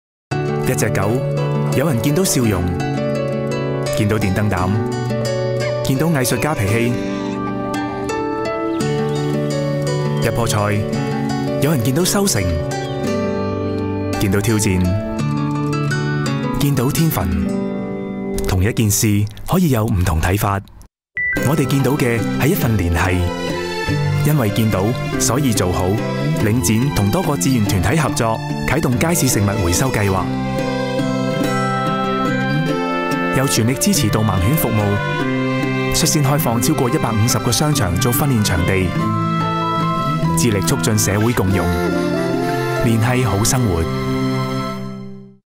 The Link 'Guide Dog' DDB Group Hong Kong
At the heart of DDB Group Hong Kong’s 2017 campaign for The Link are two 30-second TV spots. ‘Guide Dog’ explores the different roles of dogs in our lives, not the least of which is helping the visually impaired maintain their independence, and navigate the world around them. Link has been working with the charity organisation since 2014, including sponsoring programmes to raise public awareness of guide dogs, train puppies, establish breeding and training programmes, and maintain dogs during the matching process.